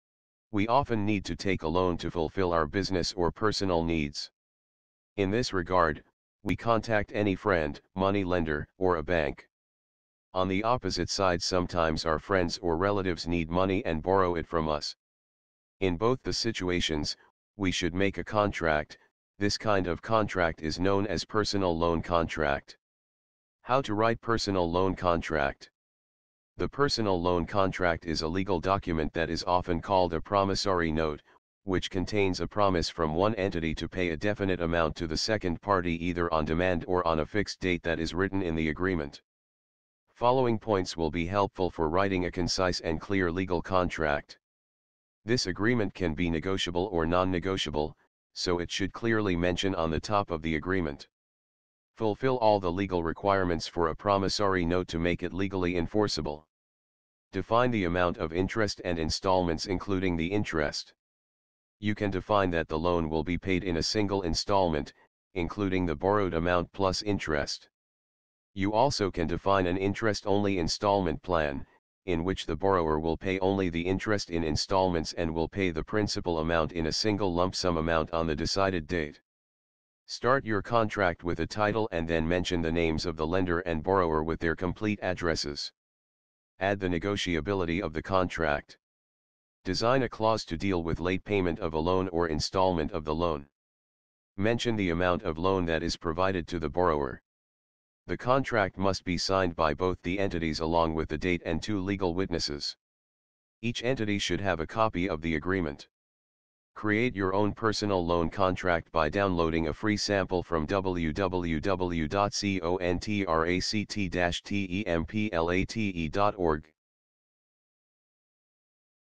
How to Write a Personal Loan Contract
Learn how to create or write a Personal Loan Contract.